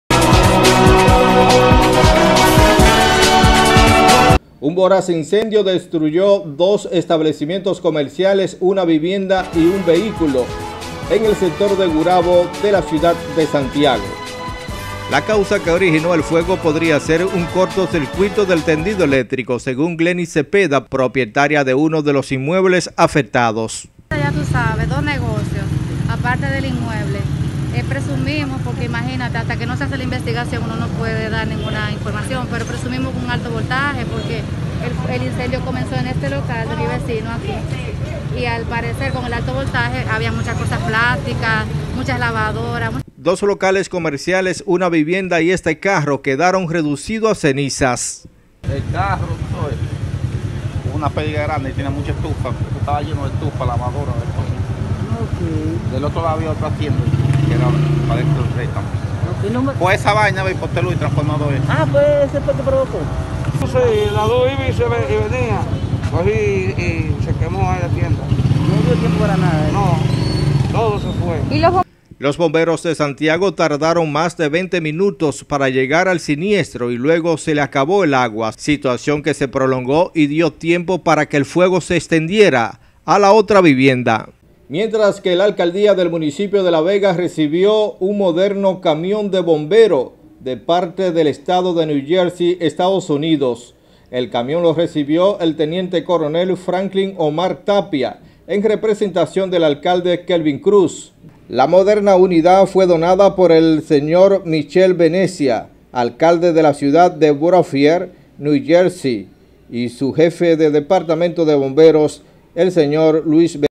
Voraz incendio en el sector de Gurabo - Santiago
Rafael Martinez │ Periodista y Productor